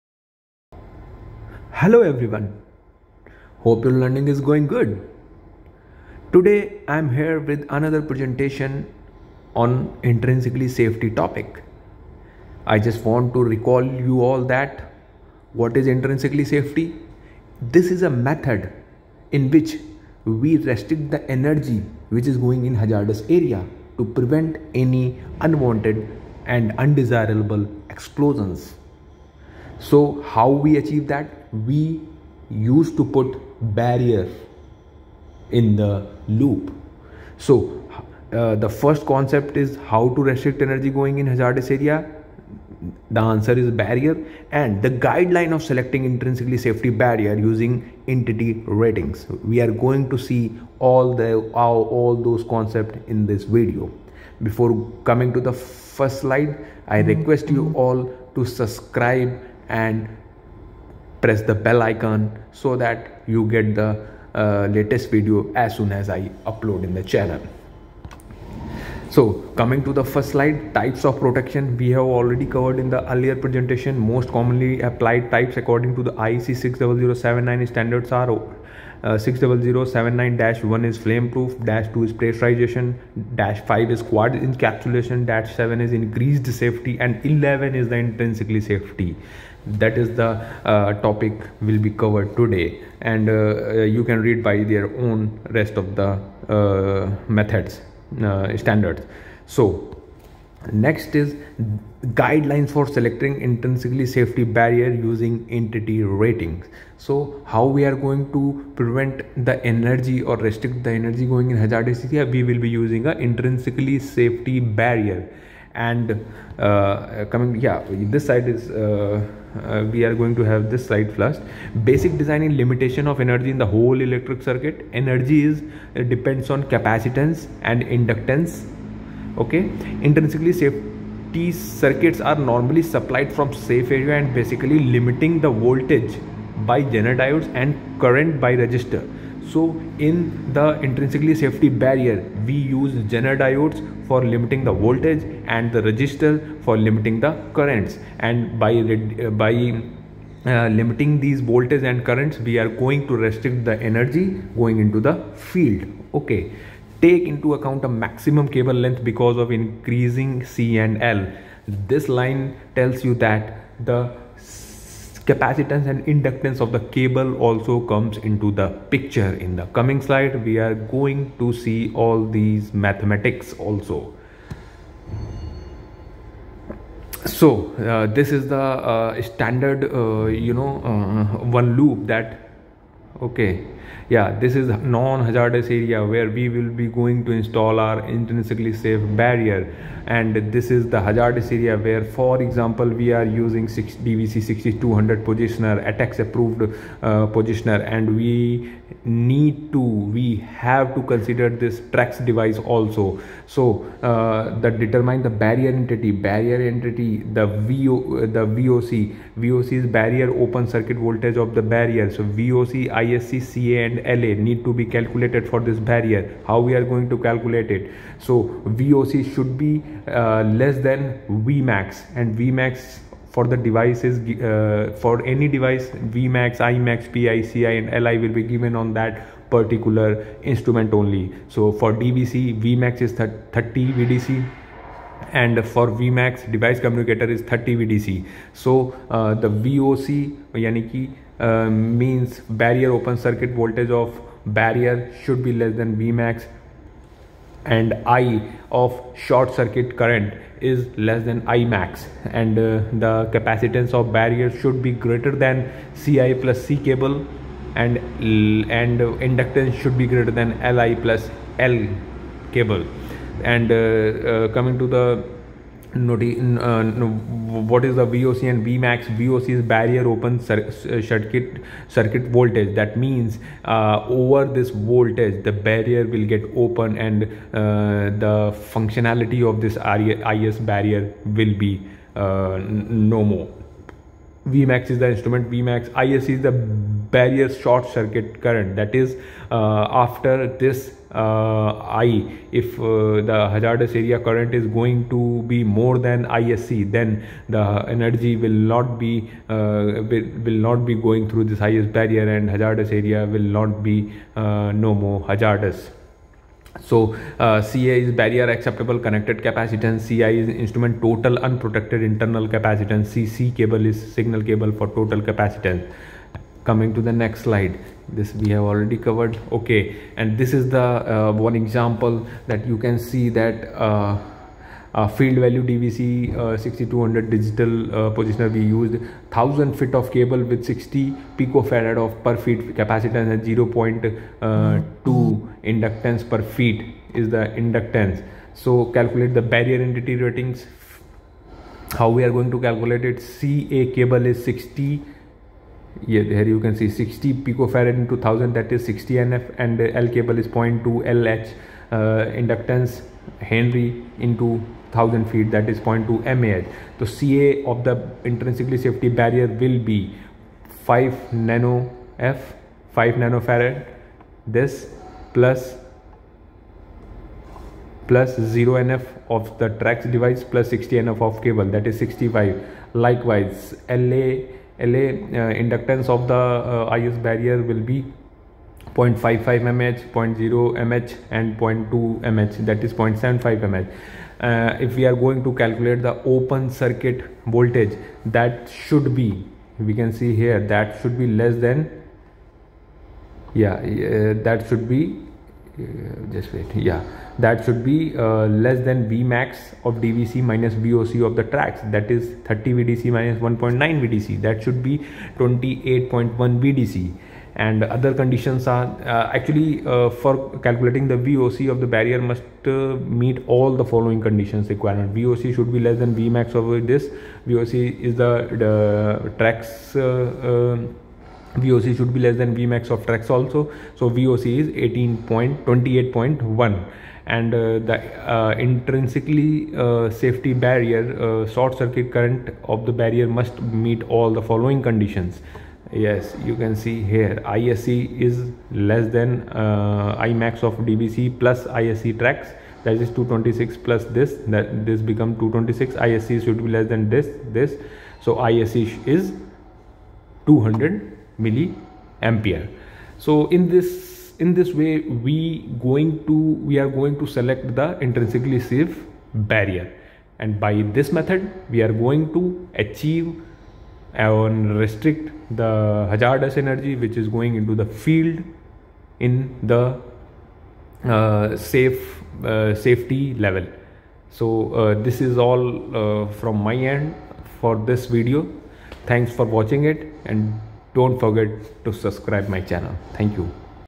Intrinsically safety IEC60079-11
Intrinsic Safety (IS) is an approach to the design of equipment going into hazardous areas. The idea is to reduce the available energy to a level where it is too low to cause ignition. That means preventing sparks and keeping temperatures low.